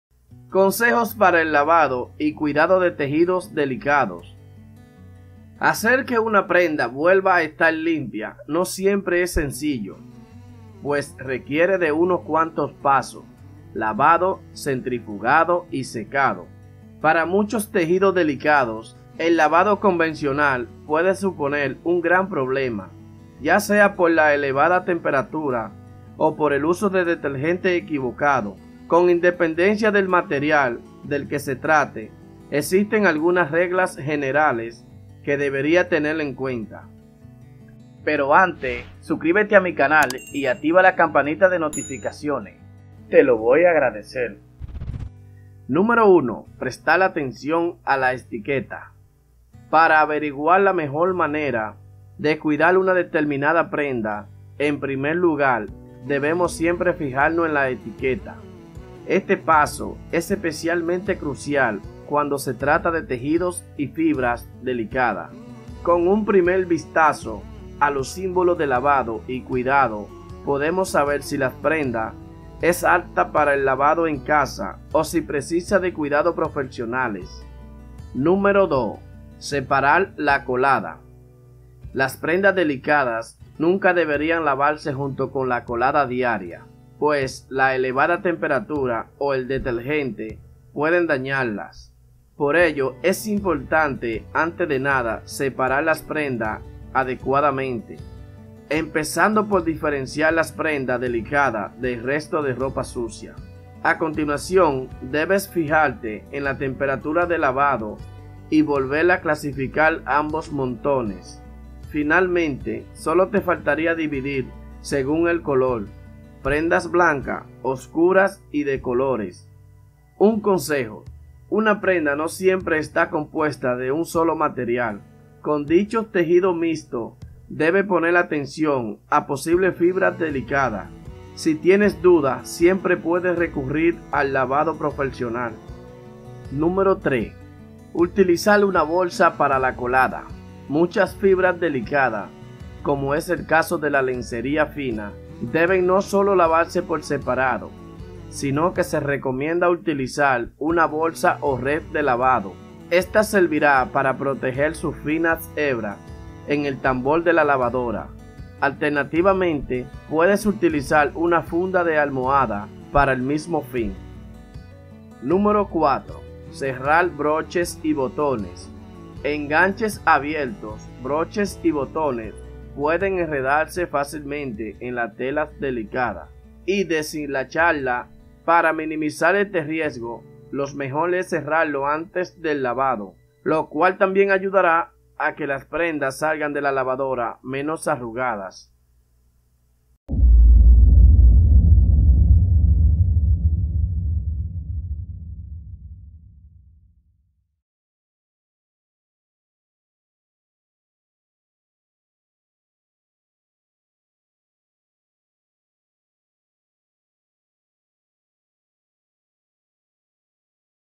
Consejos para el lavado y cuidado de tejidos delicados
Hacer que una prenda vuelva a estar limpia no siempre es sencillo, pues requiere de unos cuantos pasos: lavado, centrifugado y secado.
Para muchos tejidos delicados, el lavado convencional puede suponer un gran problema, ya sea por la elevada temperatura o el uso del detergente equivocado. Con independencia del material del que se trate, existen algunas reglas generales que deberías tener en cuenta.

1. Prestar atención a las etiquetas
Para averiguar la mejor manera de cuidar una determinada prenda, en primer lugar, debemos siempre fijarnos en la etiqueta.
Este paso es especialmente crucial cuando se trata de tejidos y fibras delicados: con un primer vistazo a los símbolos de lavado y cuidado podemos saber si la prenda es apta para el lavado en casa o si precisa cuidados profesionales.

2. Separar la colada
Las prendas delicadas nunca deberían lavarse junto con la colada diaria, pues la elevada temperatura o el detergente pueden dañarlas. Por ello es importante antes de nada separar las prendas adecuadamente, empezando por diferenciar las prendas delicadas del resto de ropa sucia.
A continuación, debes fijarte en la temperatura de lavado y volver a clasificar ambos montones.
Finalmente solo te faltaría dividir según el color: prendas blancas, oscuras y de colores.
Un consejo: una prenda no siempre está compuesta de un solo material. Con dichos tejidos mixtos debes poner atención a posibles fibras delicadas: si tienes dudas, siempre puedes recurrir al lavado profesional.

3. Utilizar una bolsa para la colada
Muchas fibras delicadas, como es el caso de la lencería fina, deben no solo lavarse por separado, sino que se recomienda utilizar, una bolsa o red de lavado.
Esta servirá para proteger sus finas hebras en el tambor de la lavadora. Alternativamente, puedes utilizar una funda de almohada para el mismo fin.

4. Cerrar broches y botones
Enganches abiertos, broches y botones pueden enredarse fácilmente en las telas delicadas y deshilacharlas. Para minimizar este riesgo, lo mejor es cerrarlos antes del lavado, lo cual también ayudará a que las prendas salgan de la lavadora menos arrugadas.